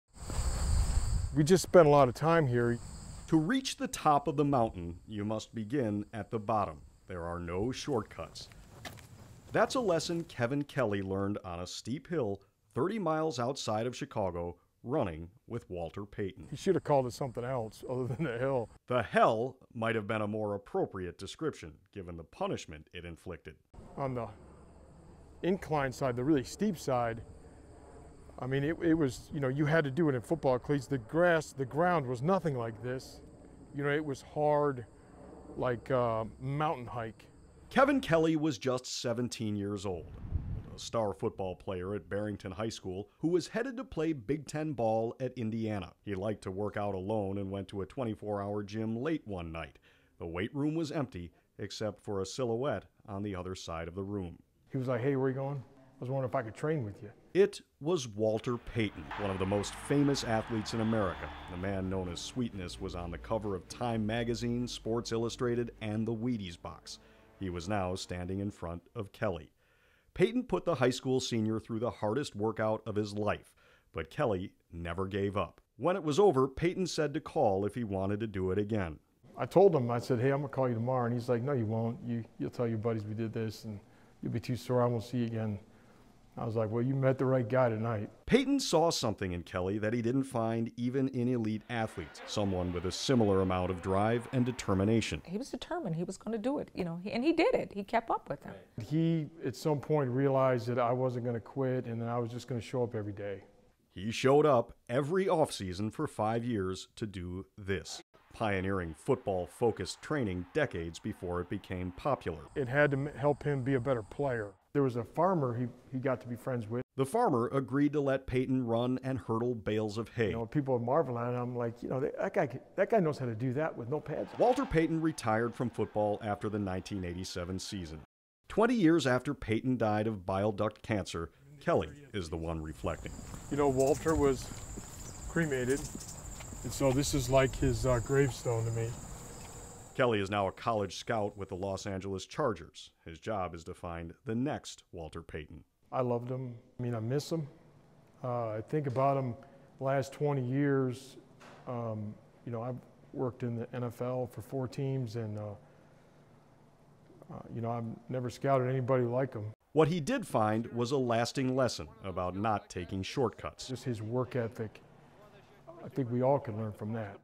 How a teen athlete became Walter Payton's training partner on 'The Hill'
On a steep hill some 30 miles northwest of Chicago, Kevin Kelly learned a lesson from Walter Payton about the value of hard work.